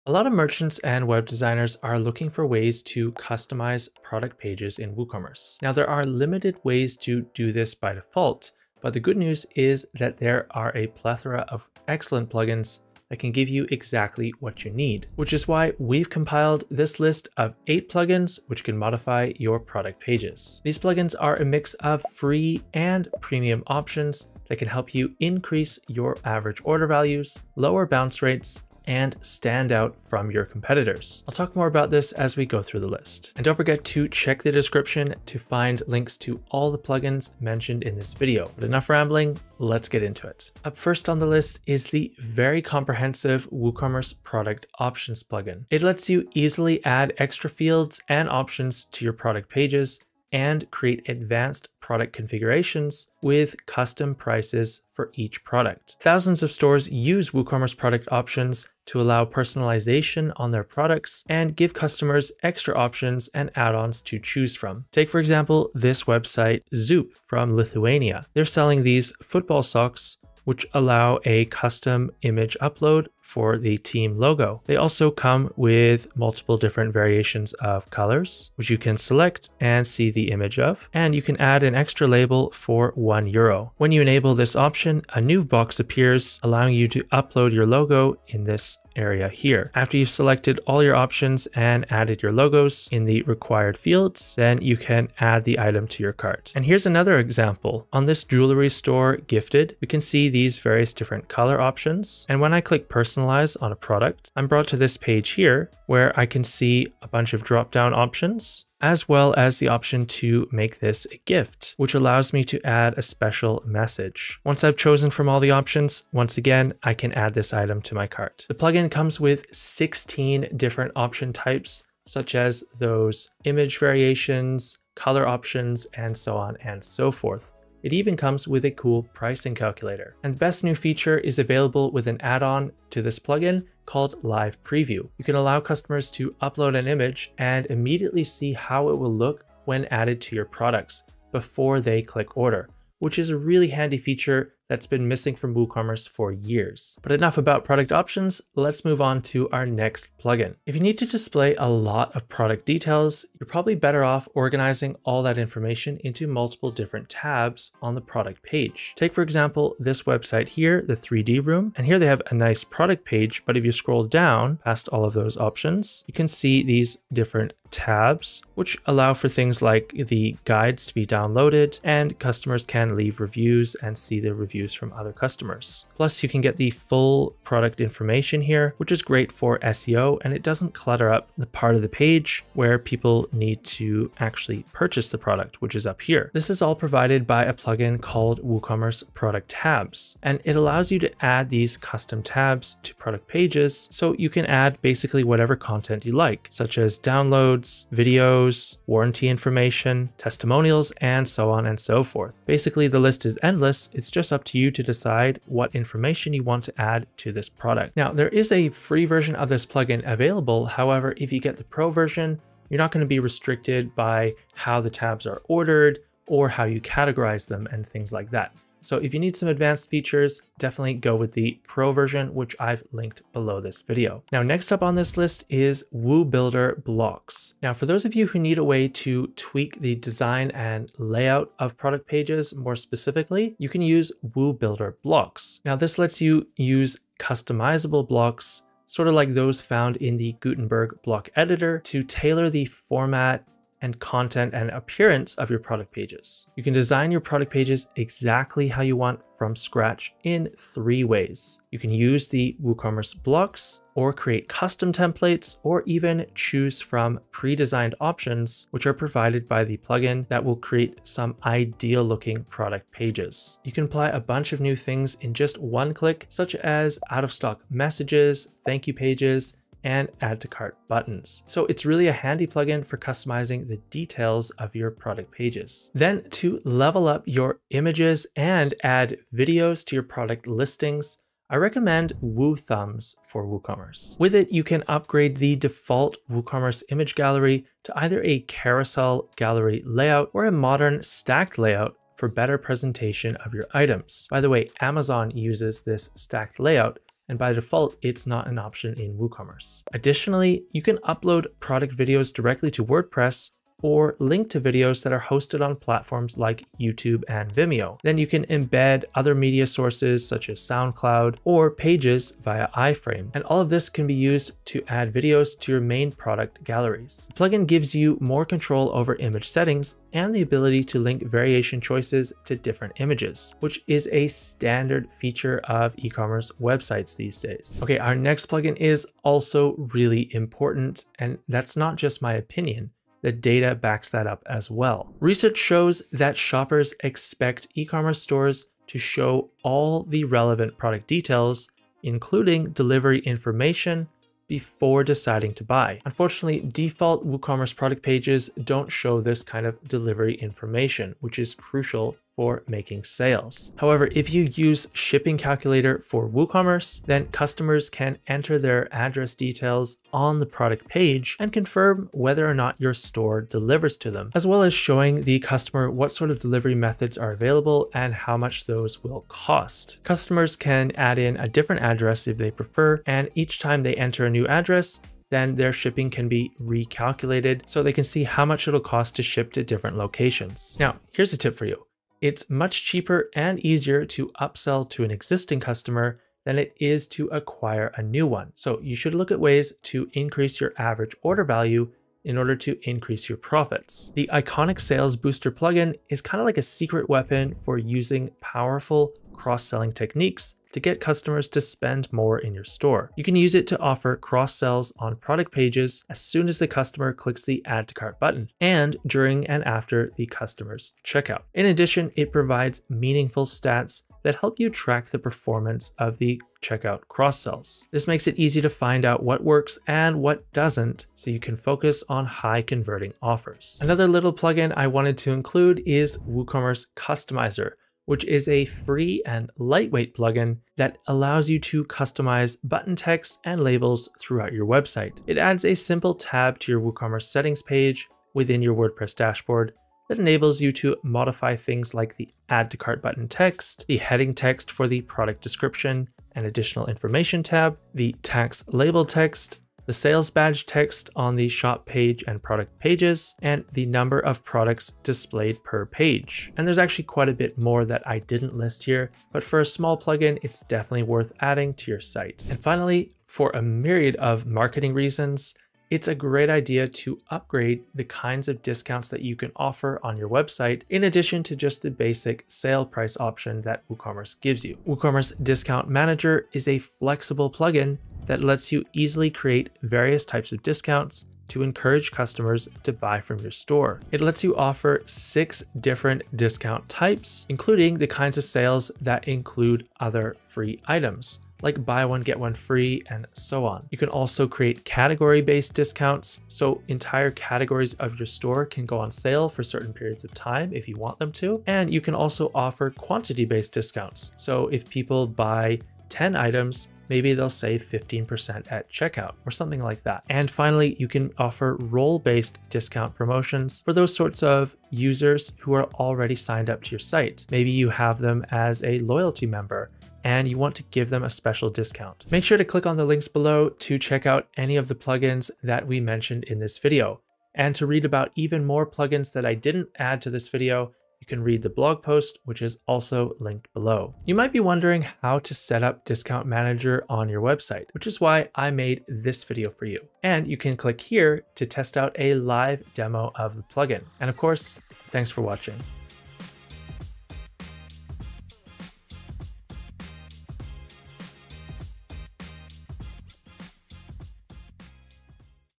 Top 8 Plugins To Modify Your WooCommerce Product Page
Looking for ways to customize product pages in WooCommerce? Lucky for you, we’ve compiled a list of the top 8 plugins that can modify your product pages.

There are limited ways to customize the WooCommerce product page by default, but the good news is that there are plenty of handy plugins that can give you exactly what you need! These plugins are a mix of free and premium options that can help you to increase your average order values, lower bounce rates, and stand out from your competitors. I’ll talk more about this as we go through the list.

Video Chapters:
0:00 Can you customize product pages?
0:40 Plugin 1
2:30 Plugin 2
3:44 Plugin 3
4:36 Plugin 4
5:27 Plugin 5
6:20 Plugin 6
7:03 Plugin 7
7:41 Plugin 8
8:41 Final tips